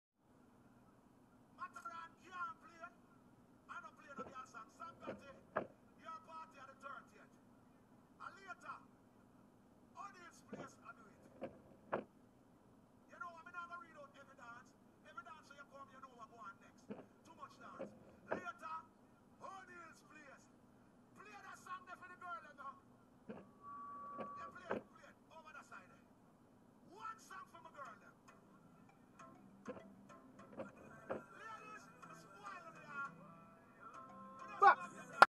accident at 2mph
chevy rear ends civic after no paying proper attention to the traffic movement and signal ahead..probably texting who knows